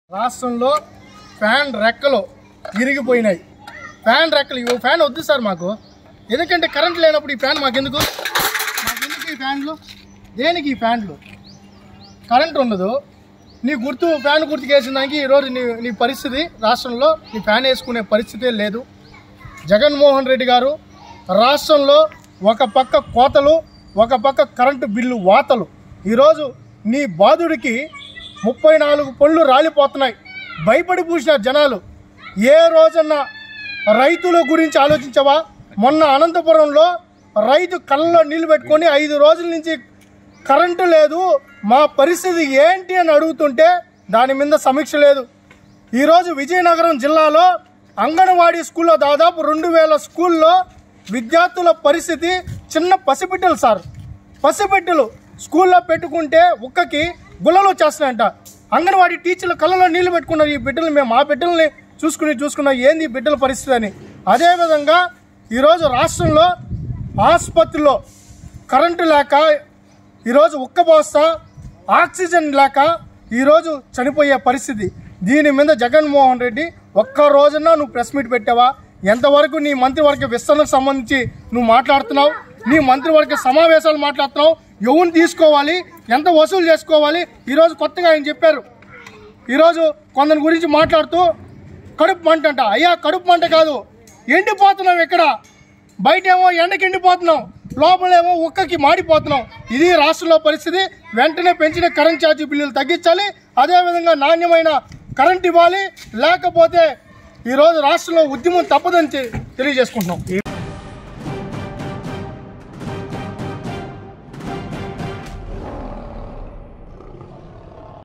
TDP Youth Leader Ravi Naidu Satirical Comments on CM Jagan | Leo News
Leo Politics

Leo Health

Leo Entertainment

Leo Devotional